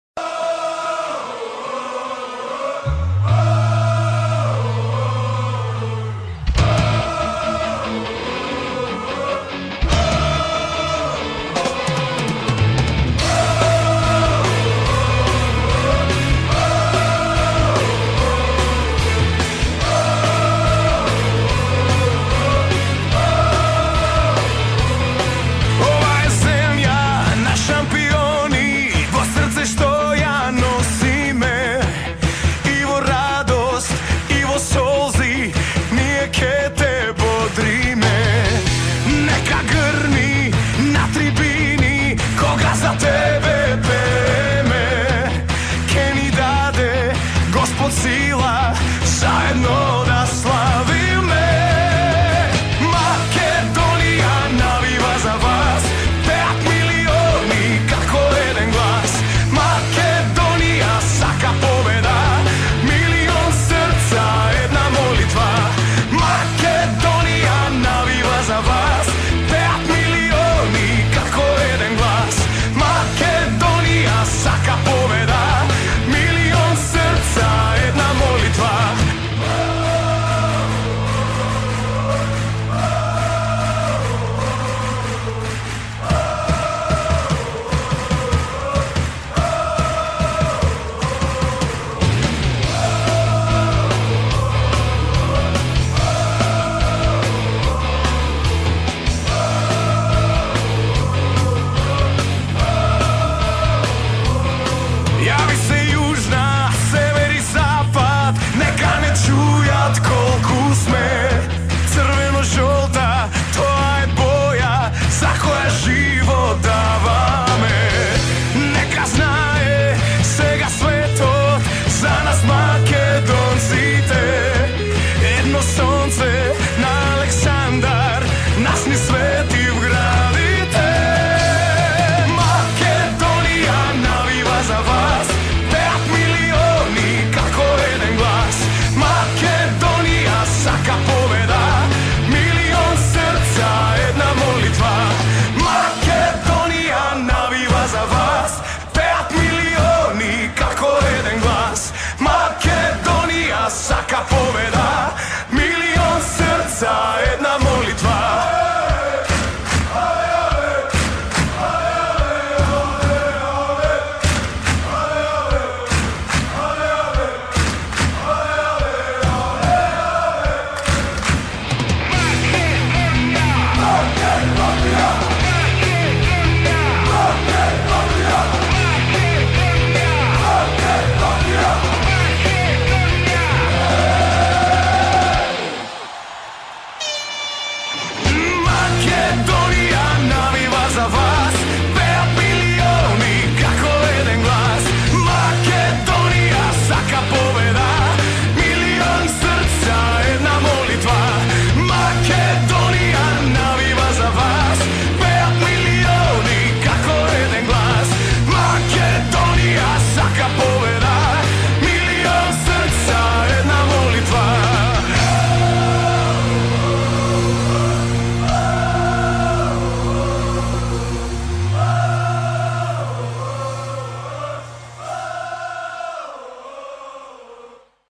Dođi da zajedno navijamo za Tošu ( 16.10.2012 Makedonija Srbija )
Nije bitno ko će pobediti Makedonija ili Srbija. Bitno je da se te večeri setimo Tošeta Proeskog. Koji je mnogo toga uradio za Makedoniju, Srbiju i ostale države bivše
Yugoslavije